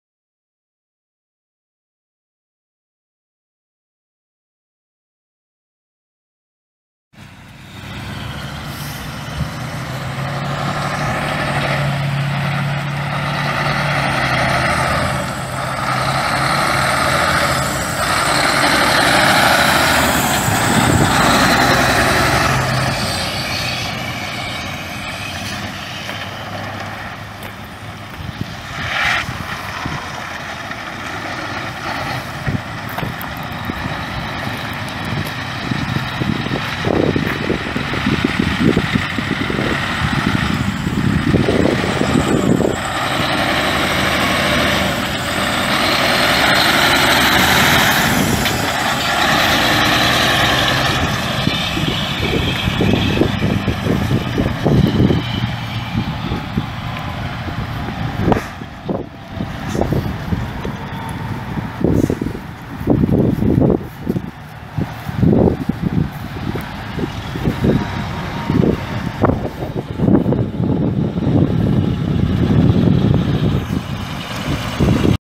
2006 Volvo VNL semi truck for sale | no-reserve Internet auction May 4, 2017
2006 Volvo VNL semi truck
955,031 miles on odometer
Volvo VE D12 12.1L L6 turbo diesel engine
Automatic transmission
Model RTO16810B-AS
Tandem axle
AM/FM/CD
Heated mirrors
Air suspension seat
Sleeper
Double bunk
Storage cabinets
Rear climate control
Hanging closet
Air suspension
Air brakes
275/80R22.5 front tires
445/50R22.5 rear tires
GVWR 50,000 lbs
Broken hood hinges
Missouri title